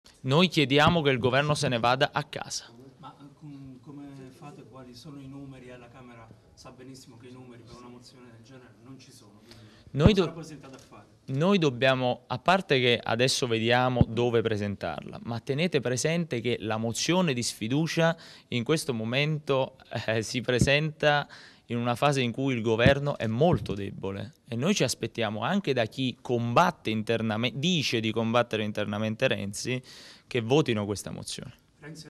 SFIDUCIA AL GOVERNO, DI MAIO ALLA MINORANZA PD: VOTATE CON NOI M5S
(Agenzia VISTA) - Roma, 1 Aprile 2016 - Il vicepresidente della Camera dei Deputati, Luigi di Maio, a margine di un convegno M5S su intelligence e sicurezza presso l'Aula dei Gruppi Parlamentari.